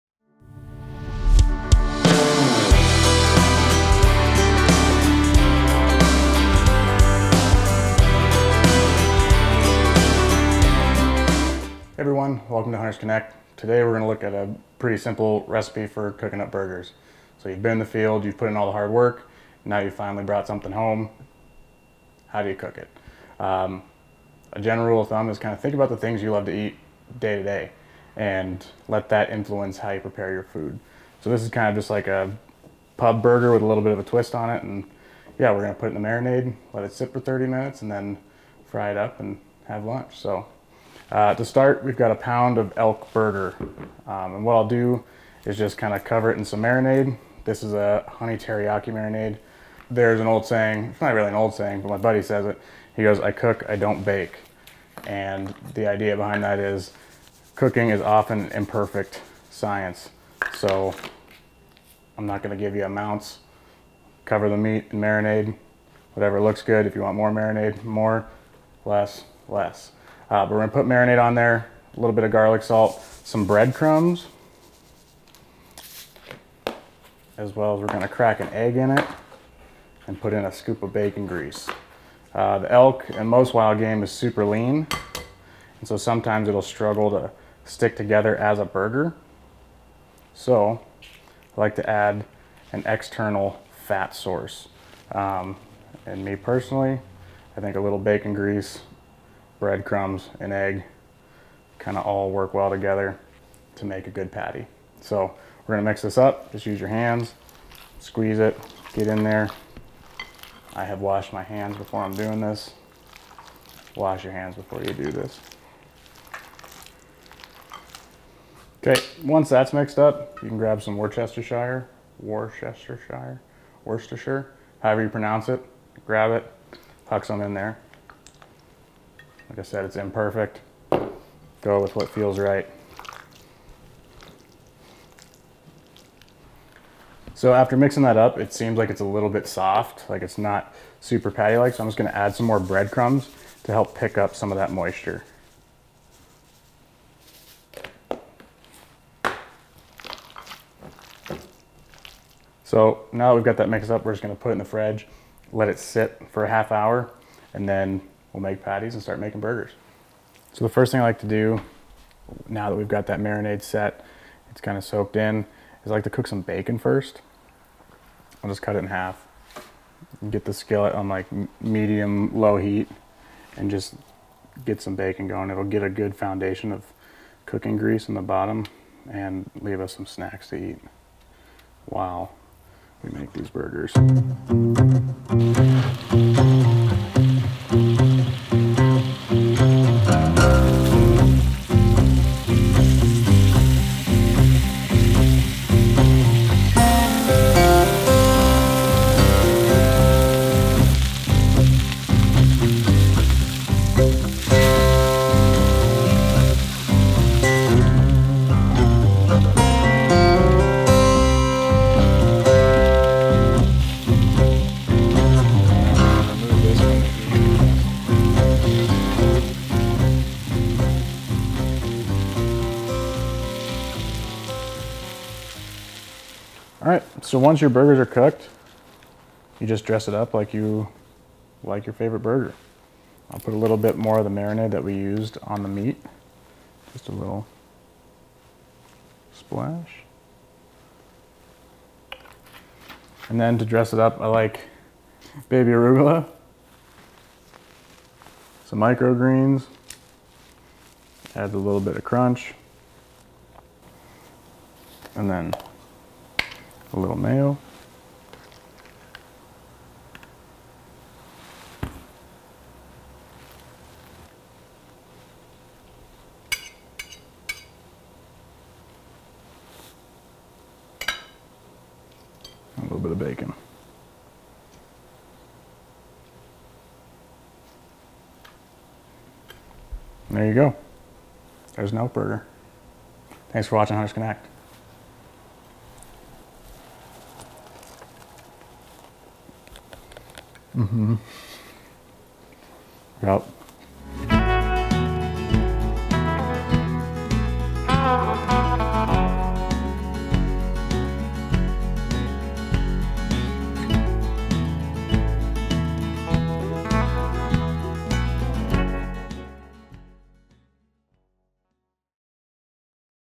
PUB STYLE ELK BURGERS | Hunters Connect
One of the biggest questions we get is how to prepare your wild game once you find success in the field. This week on Hunters Connect, Paul Kemper shows you how to make a simple and delicious wild game burger. Suitable for any red wild game meat, treat your family, friends, and yourself with a little twist on a classic pub burger.

Ingredients:
1lb. of game meat (we used elk in this video)
1 egg
Bacon (as much as you prefer) + bacon grease
Garlic salt
Teriyaki Marinade
Worchestershire
Microgreens
Baby Arugala
Cheese (of your choice: we used Muenster)

Your feedback helps us continue to help Hunters Connect.

Big thanks to our supporters!